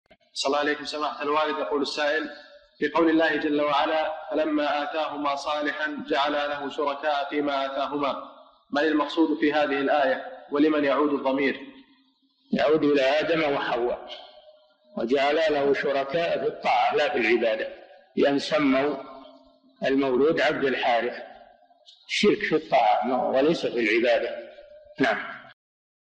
[42] لمن يعود الضمير في قوله ﷻ:﴿فلما آتاهما صالحا جعلا له شركاء فيما آتاهما﴾ ؟ - الشيخ صالح الفوزان
"[4717-42] في قول الله جل وعلا ﴿فَلَمَّا آتَاهُمَا صَالِحًا جَعَلَا لَهُ شُرَكَاءَ فِيمَا آتَاهُمَا﴾ [الأعراف: 190]، من المقصود في هذه الآية ولمن يعود الضمير؟
الجَوَابُ: يعود إلى آدم وحواء، ﴿جعلا له شركاء﴾ في الطاعة لا في العبادة، بأن سموا المولود عبد الحارث، شرك في الطاعة وليس في العبادة.

للاشتراك في قناة الشيخ صالح الفوزان
فتاوى المجالس في تفسير المفصل

للاستفسار والإقتراحات والدعم
رئيس الفريق
لمشروع كبار العلماء